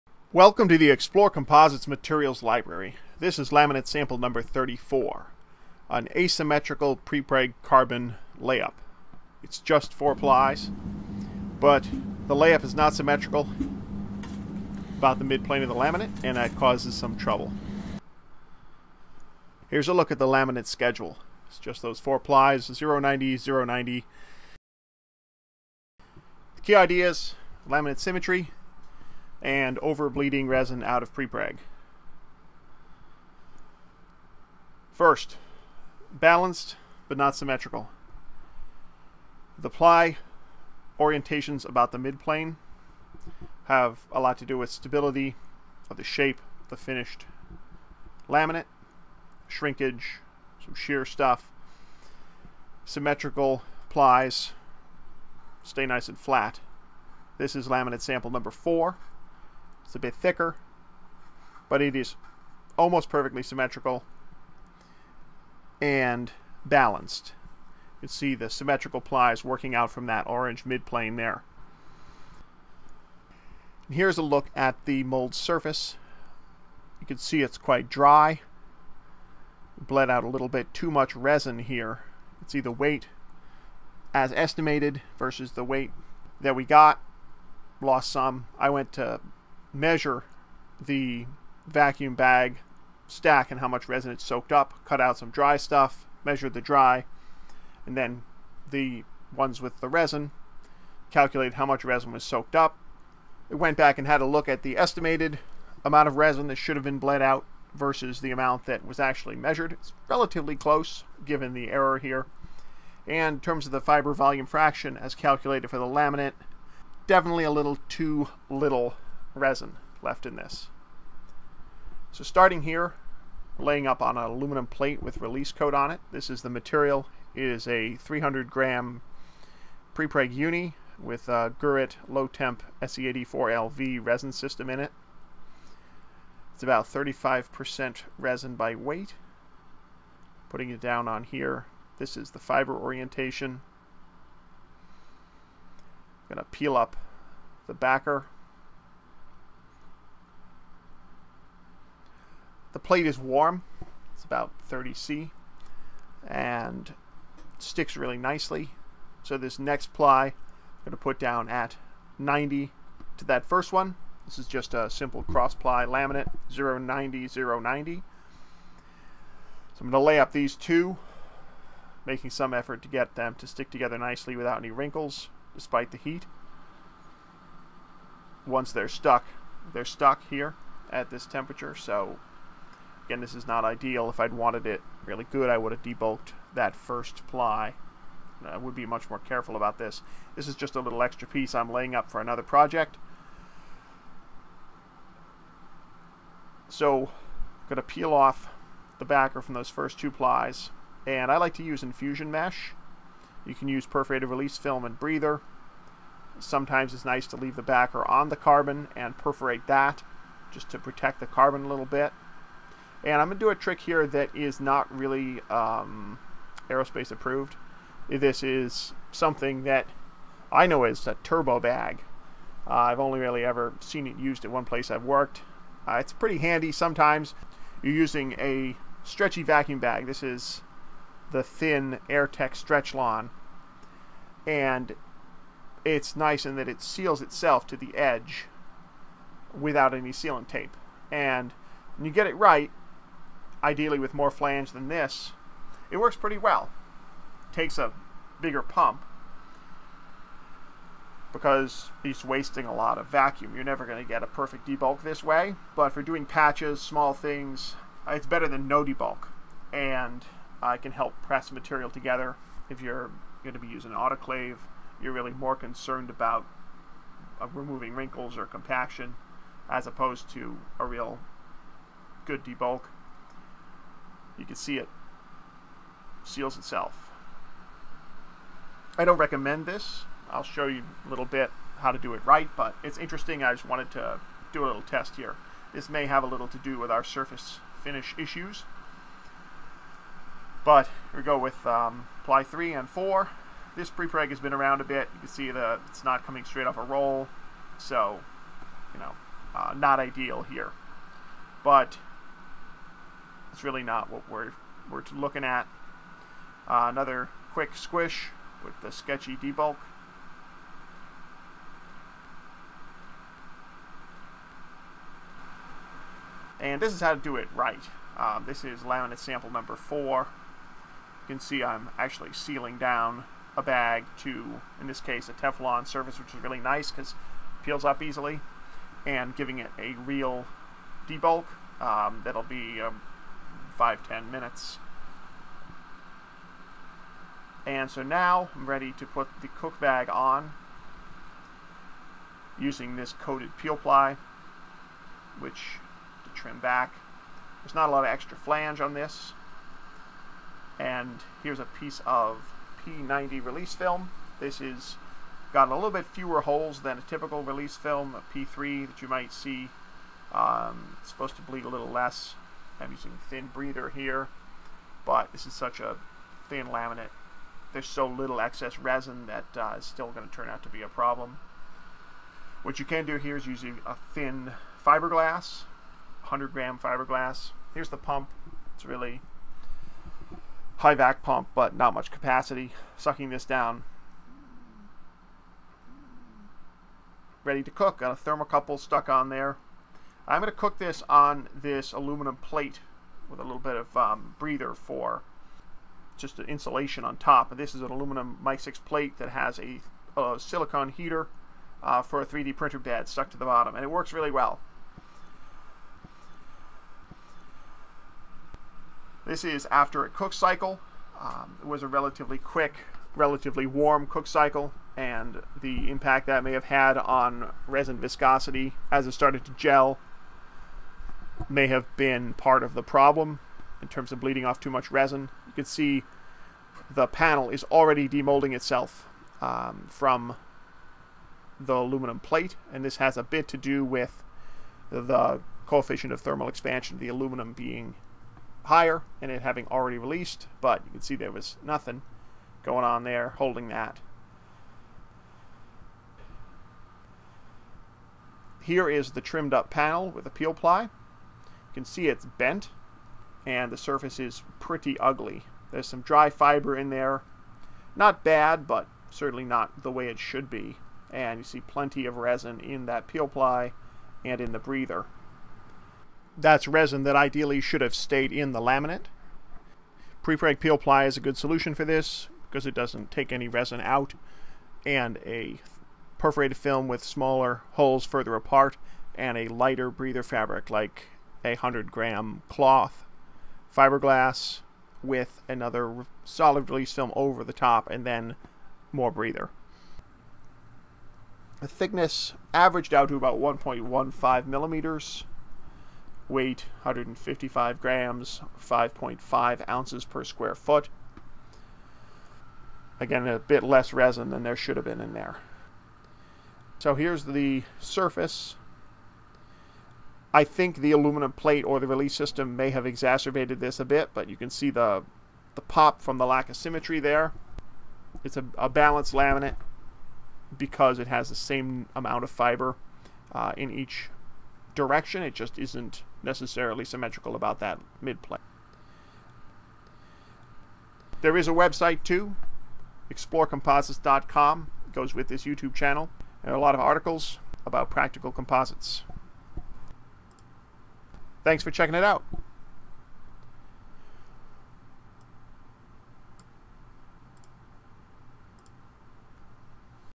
Laminate Sample #34: Asymmetric Prepreg Carbon Sheet
This Laminate Sample #34 is about asymmetric(al) laminates, resin bleed and thin prepreg.  It isn't really an example of things you SHOULD do - but more things you should avoid!

The laminate itself is four plies of 300g prepreg uni in a 0/90 "cross-ply" layup.  Because the ply orientation isn't symmetrical about the neutral axis of the laminate, there is warping when the laminate is cured.  This wouldn't happen if the stacking order had been symmetrical!

To add insult to injury, the cure cycle was too hot too fast and the resin viscosity dropped very low allowing lots of resin to bleed out.  Because there was only 35% resin or so to begin with, even a small amount of bleed reduced the resin by weight to 28% - with an estimated fiber volume fraction of 62%.  This isn't quite enough and the tool surface looks pretty dry.  Generally 30% resin by weight is a reasonable minimum for non-autoclaved prepreg.

To top it off I used a sketchy debulk method with no sealant tape on some Stretchlon film!  Don't do what I do...